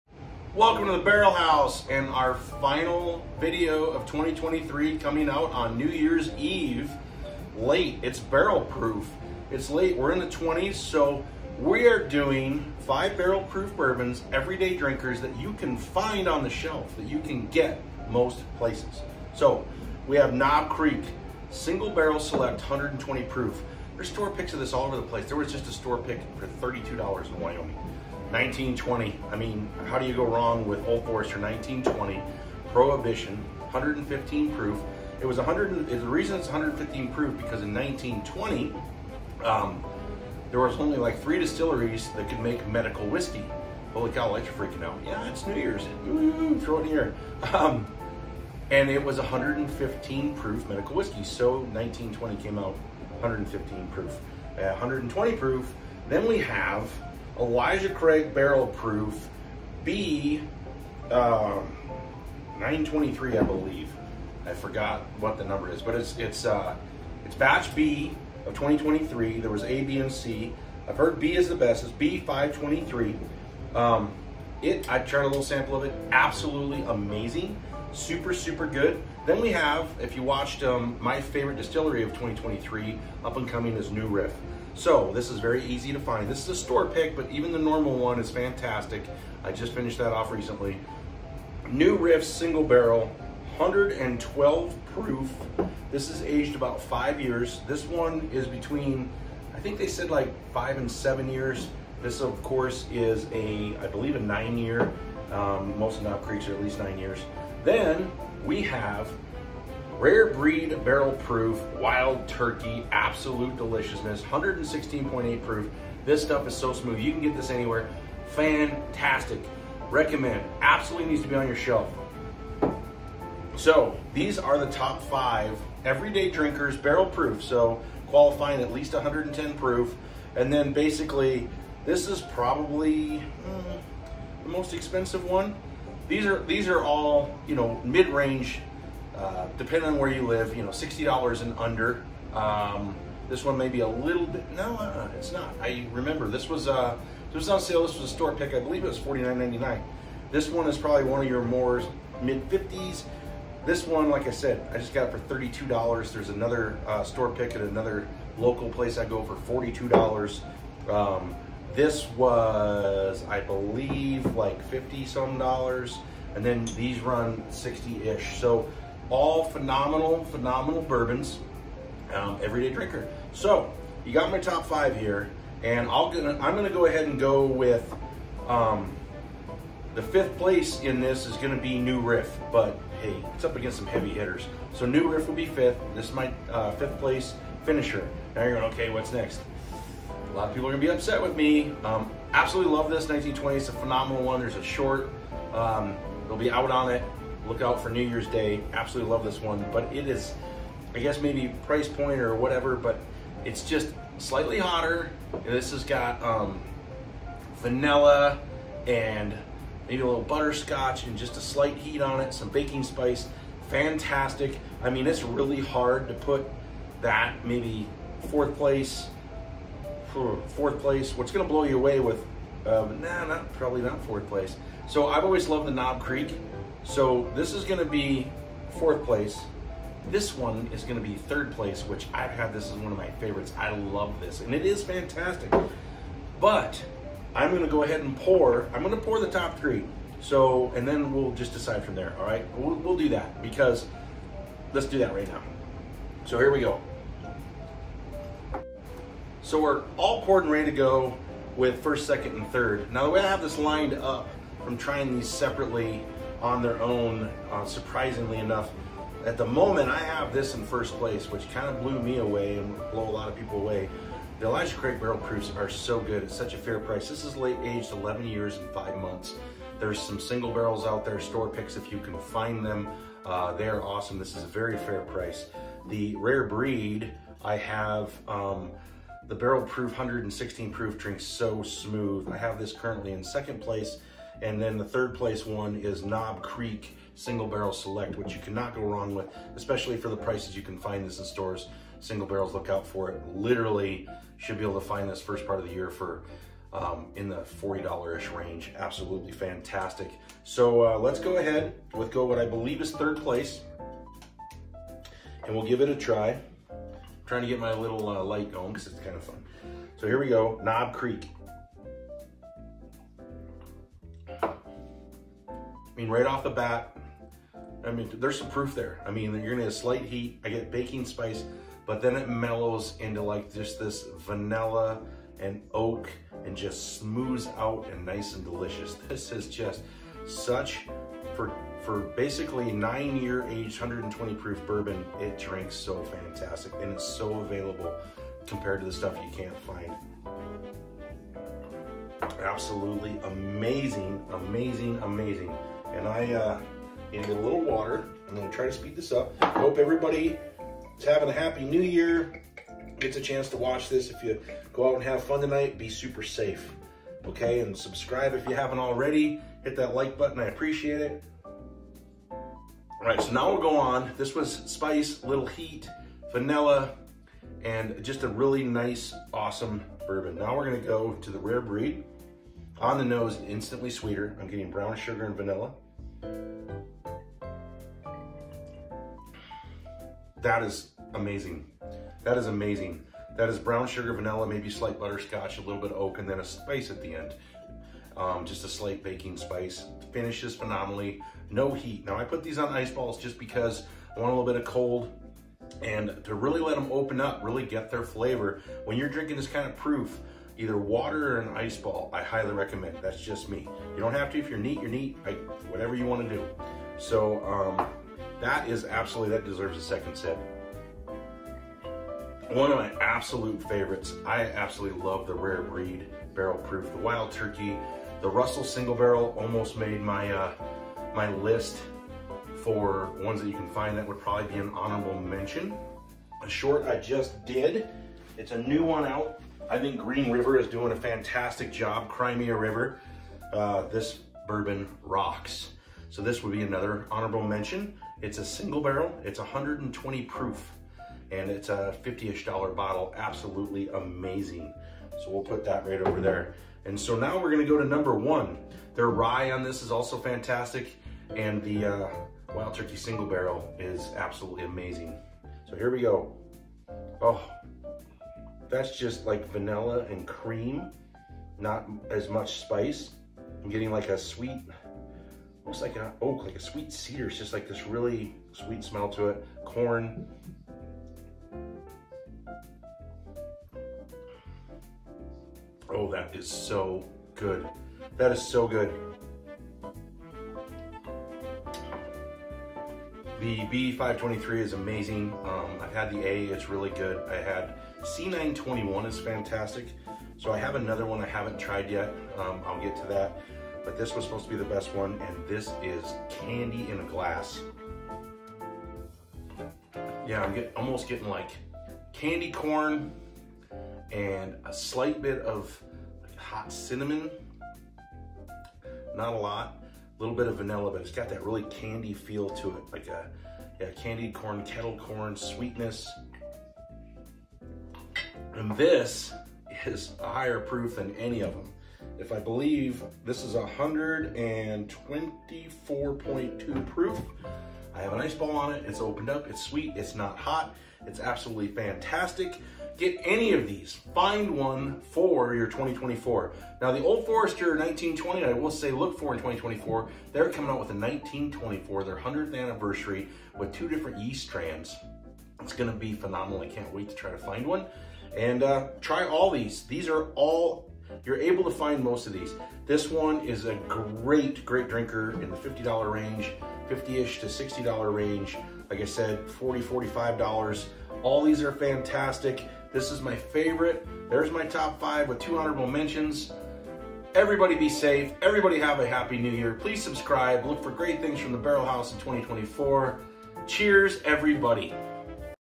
Easy to Find AMAZING BARREL PROOF’S🥃 of 2023#wildturkey
Happy new year everyone trying to top 5 every day barrel proof drinkers that you can find on your local liquor store shelves. Cheers everybody have a great 2024.